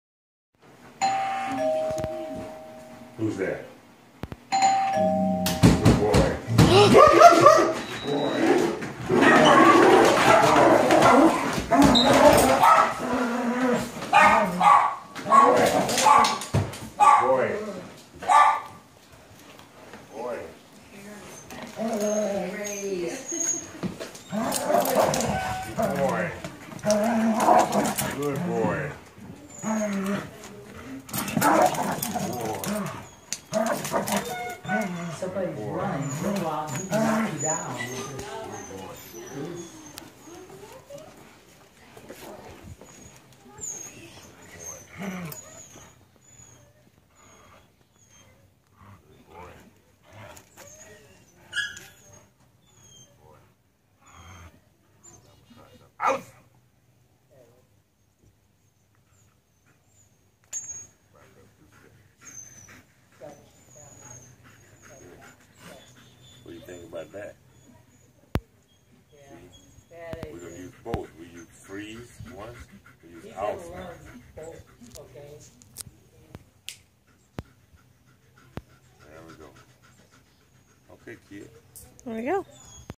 Belgian Malinois First Session for Home Protection
Belgian Malinois " Tank " First Session at Home Protection.
Trainer: Bob Smith's K9 & Decoy: Keith Black